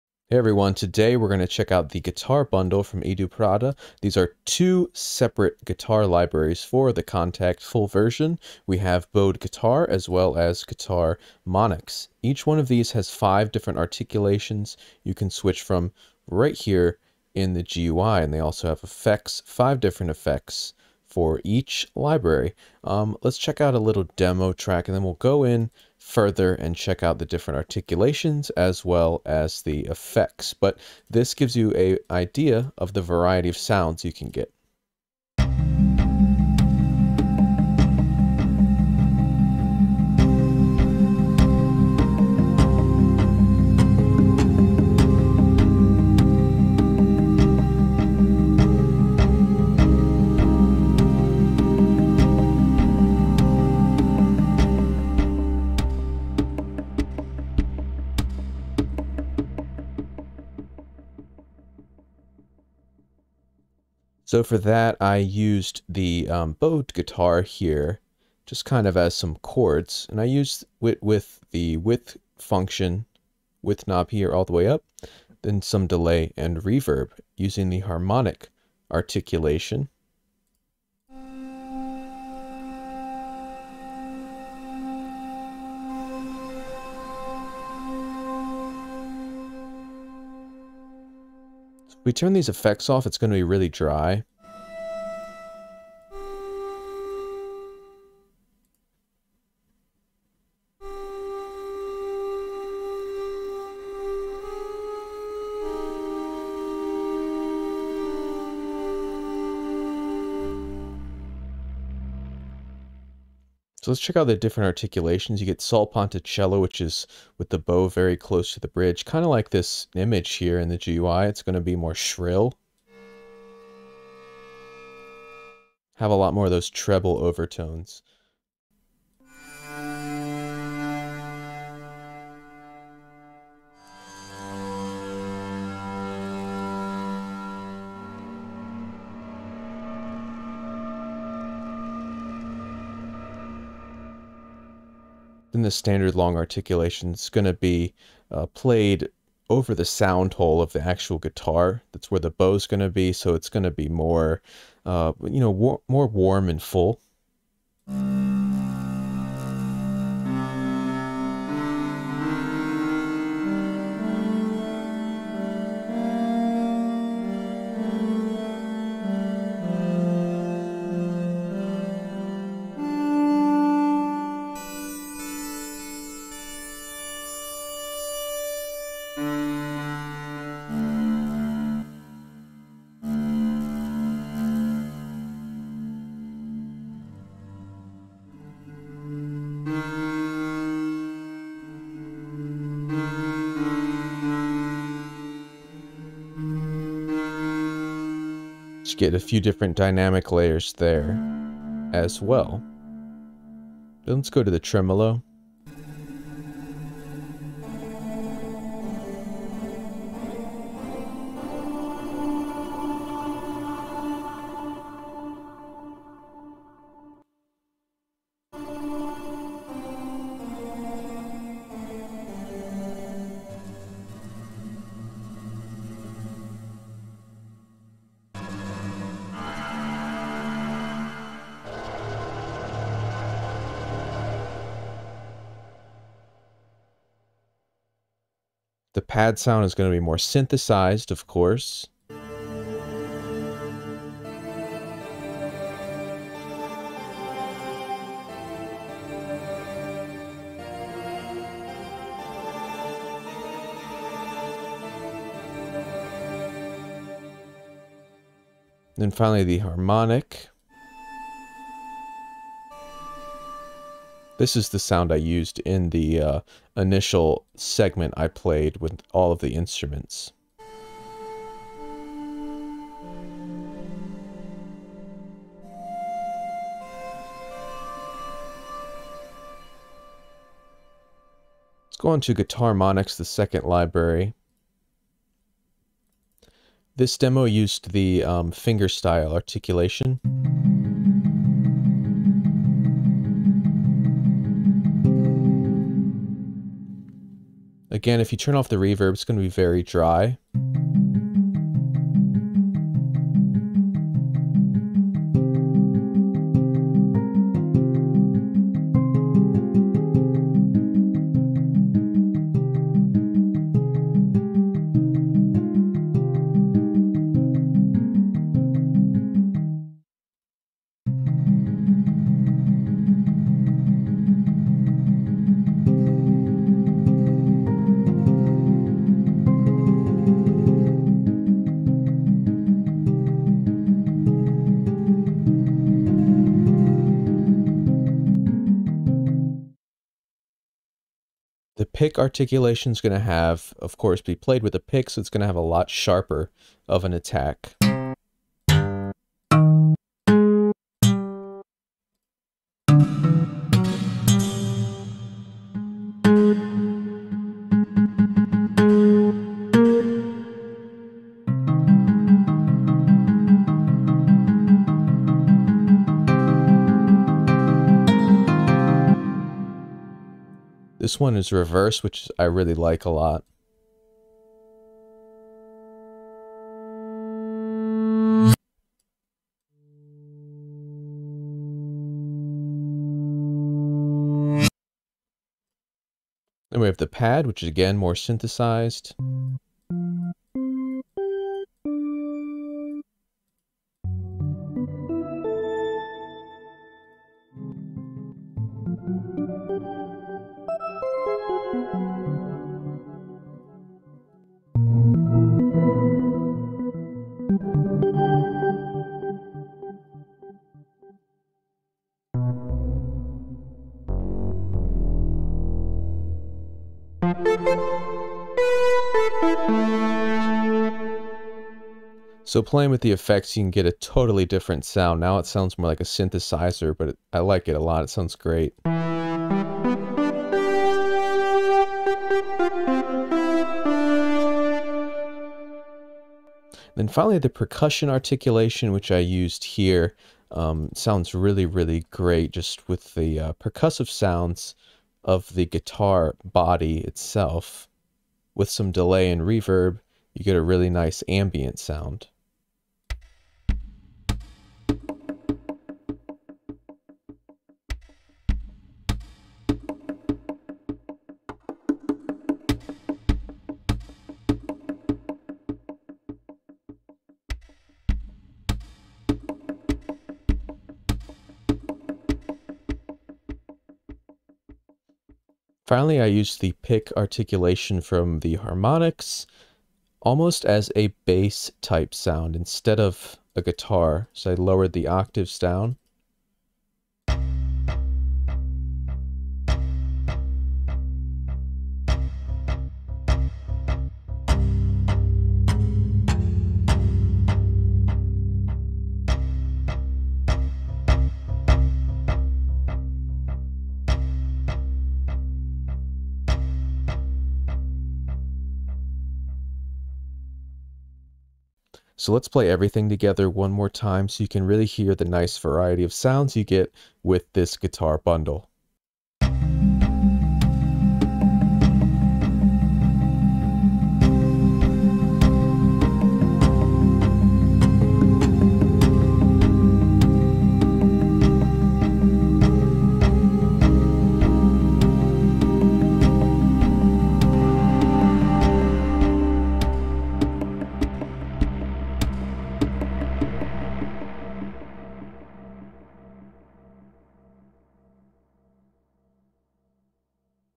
Checking out the Extended Guitar Bundle from Edu Prado!
The Guitar Bundle from Edu Prado contains two unique acoustic guitar libraries: Bowed Guitar and Guitarmonics, both requiring the Full Version of Kontakt to run. Each comes with 5 different articulations and 5 effects, which allows you to create some new and unusual sounds, as well as providing you with some unique articulations that capture the full sonic capabilities of an acoustic guitar!